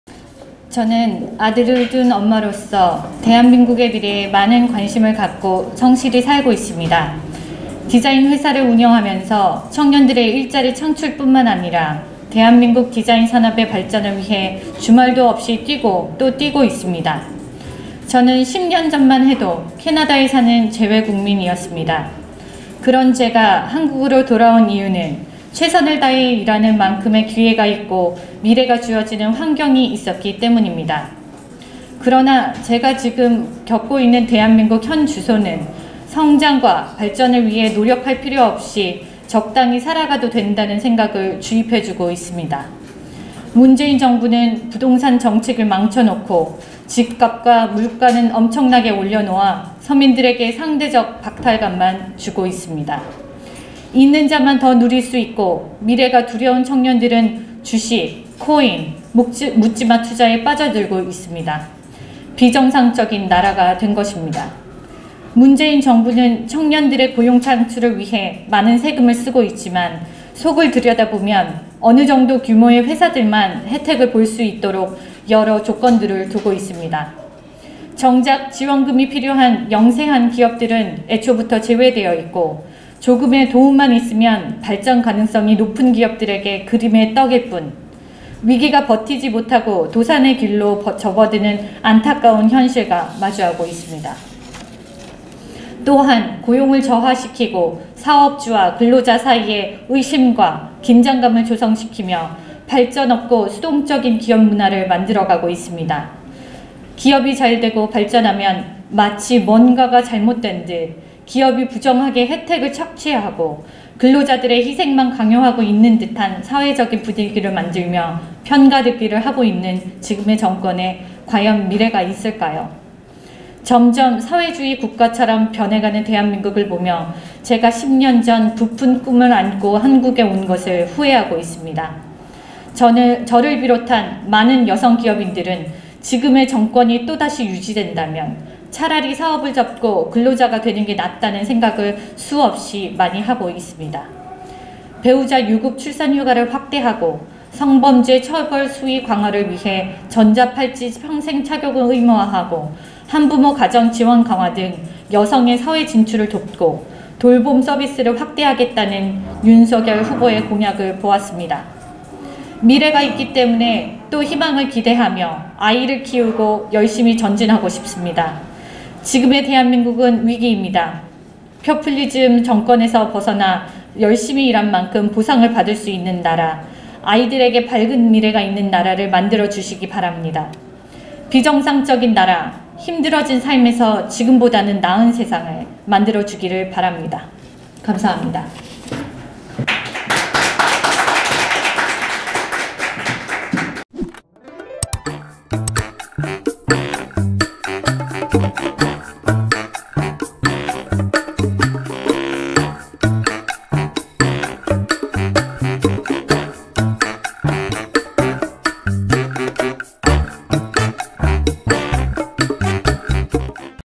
최예나 국민의힘 국민대변인 "재외국민 전 한국 온 걸 후회해...사회주의처럼 변한 한국"
국민의힘 최예나 국민대변인이 21년 12월 23일 국민의힘 중앙당사에서 논평을 브리핑 하고 있다.